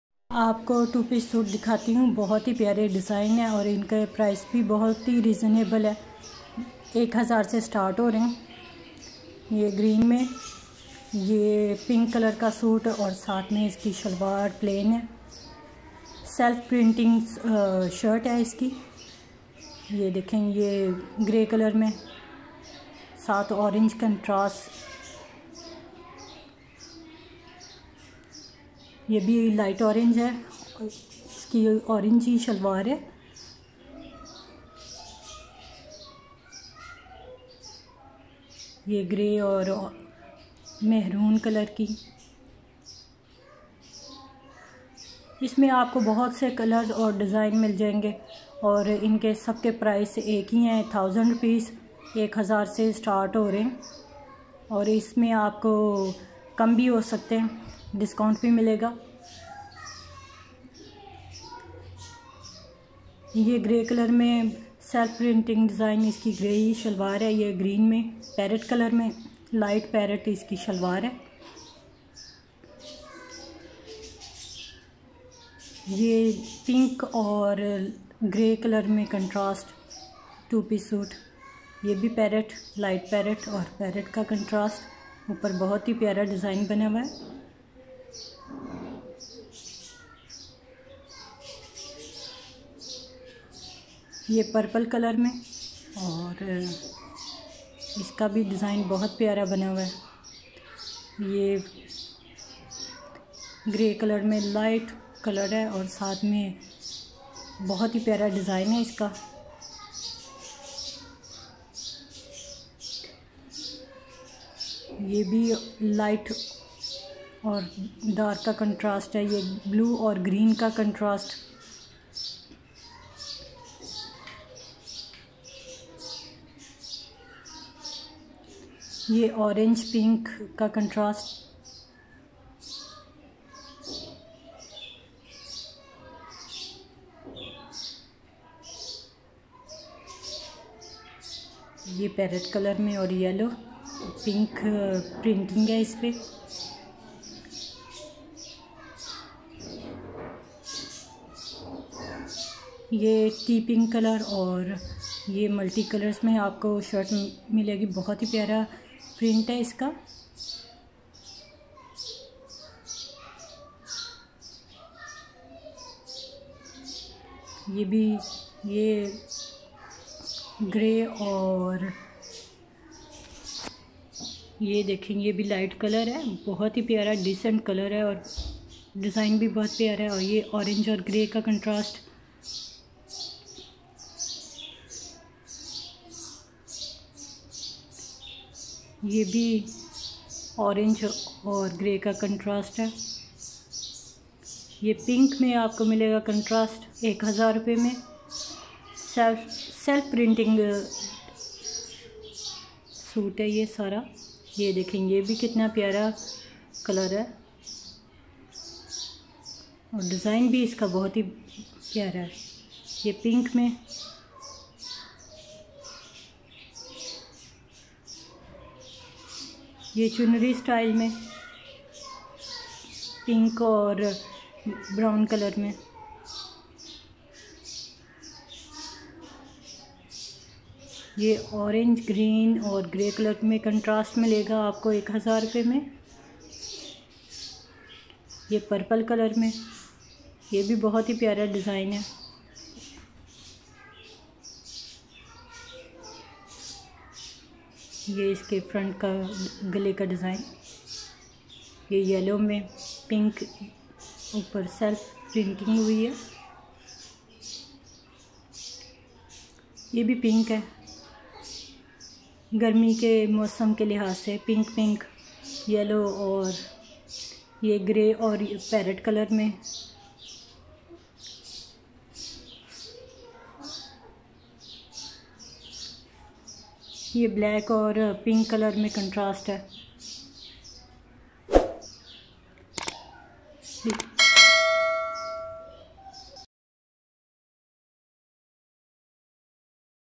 Lawn 2 piece suits Haji Ahmed Deen Cloths house
A short shopping Vlog on HAJI AHMED DEEN at BHATTA CHOWK Lahore . Have a great Sale upto 50% Unstitch, Ready to Wear & Kids accessories. Don't miss out this great opportunity.

Hope you ll enjoy shopping with me.                 Your support is greatly appreciated.